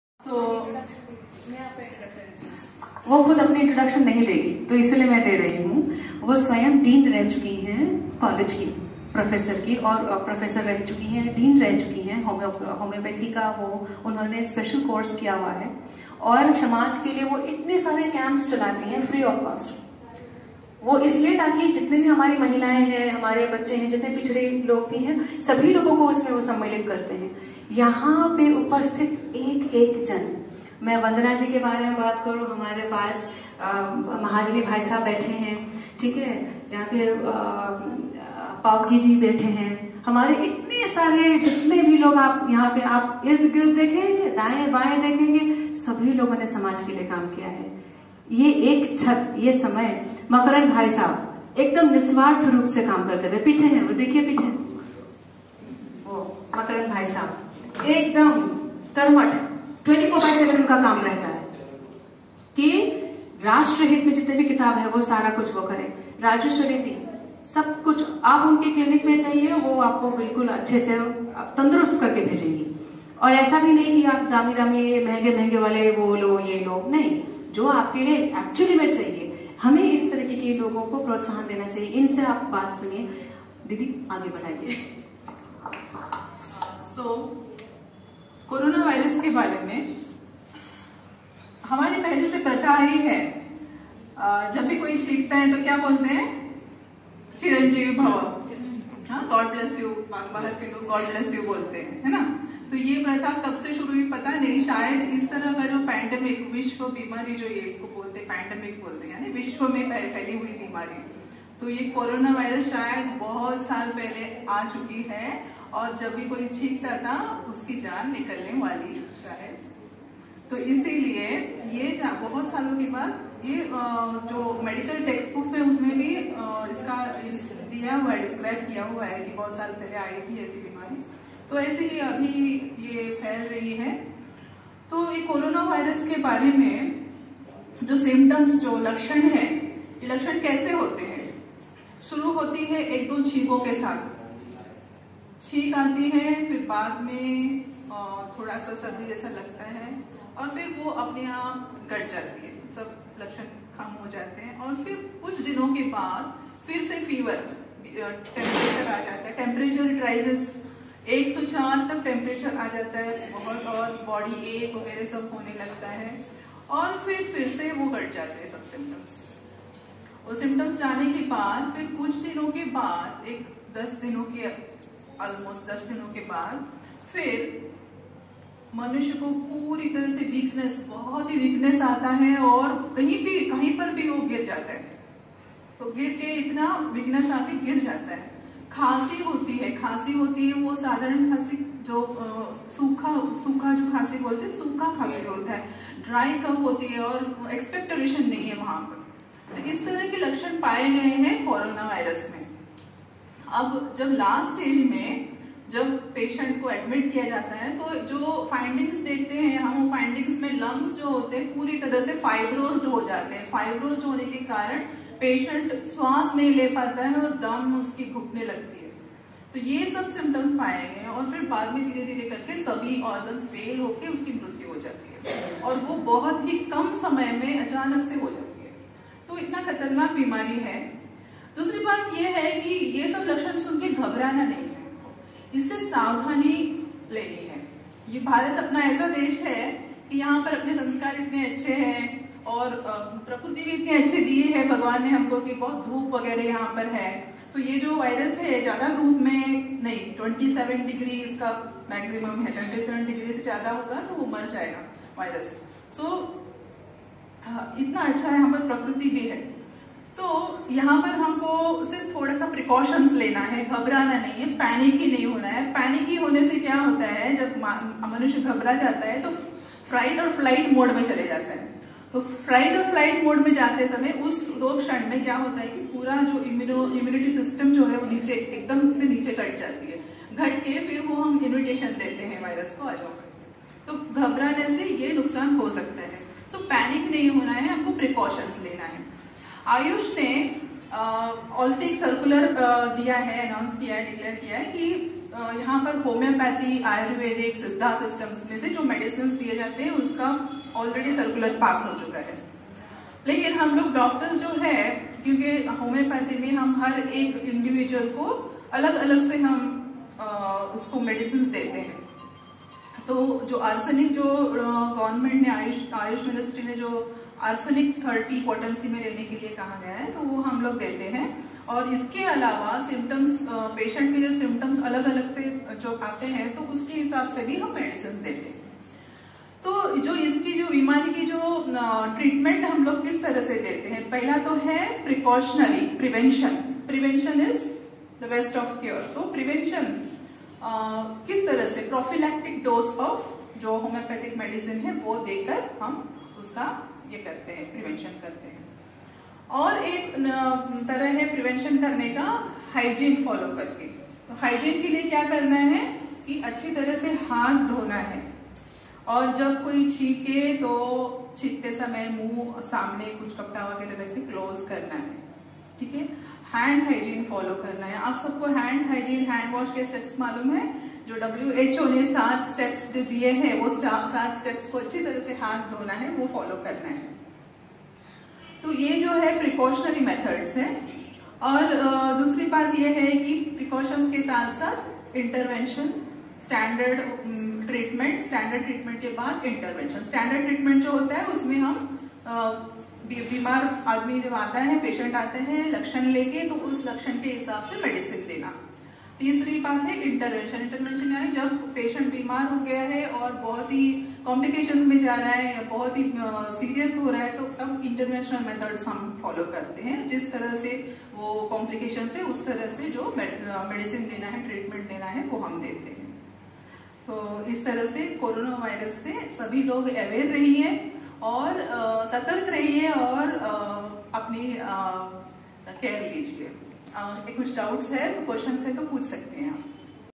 What is Corona Virus? Dr. Rajeshwari ji sharing her valuable inputs on Burning Topic
- Cover your mouth while sneezing
- Do not ignore the small symptoms
- Follow hand cleaning steps - WHO recommended
- Corona Virus is curable
- Do not panic